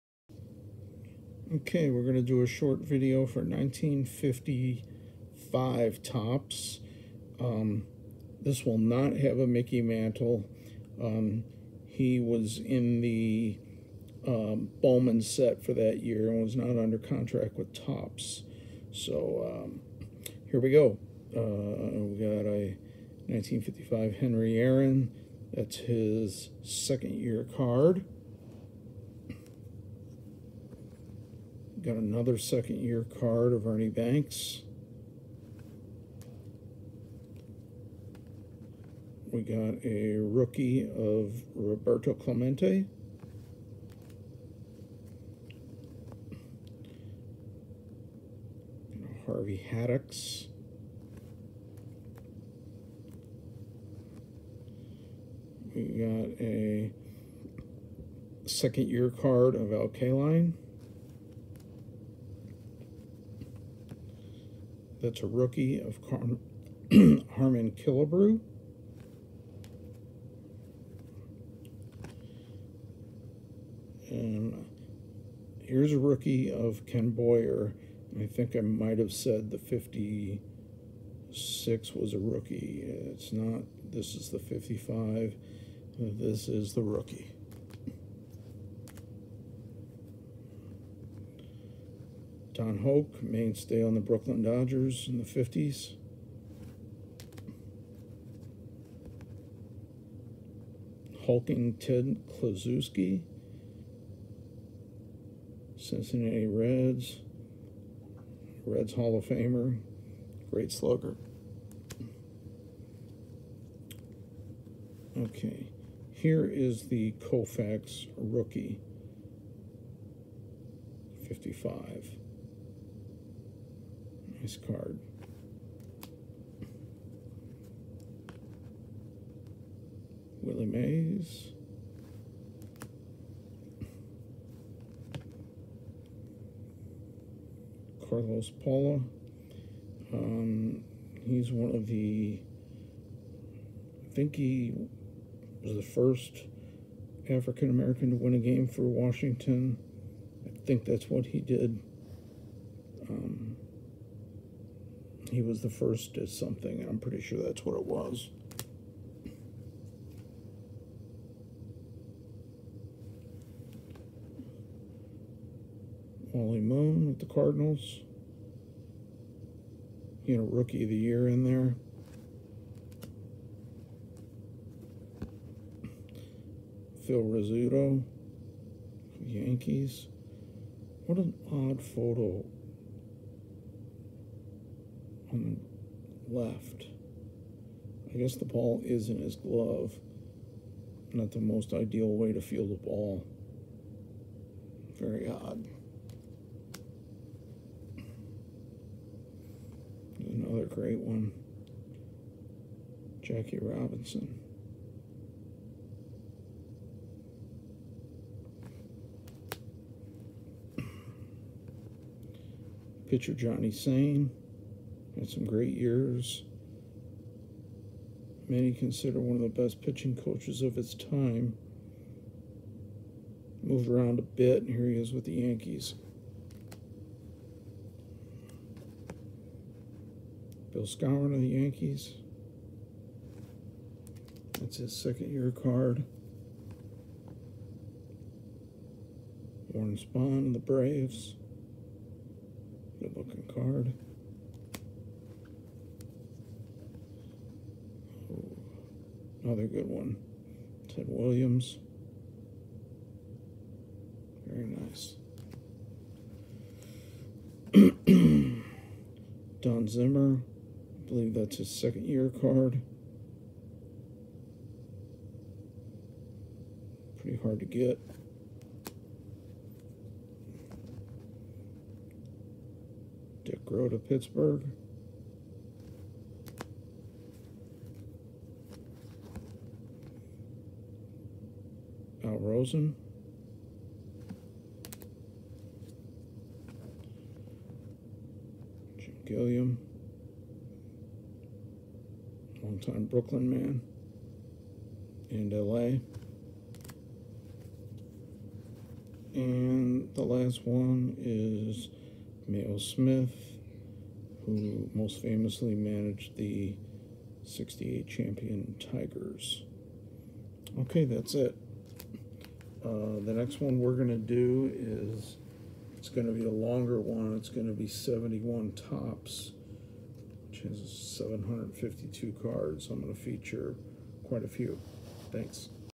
1955 Topps Baseball Superstars
Every major superstar from the 1955 Topps baseball set.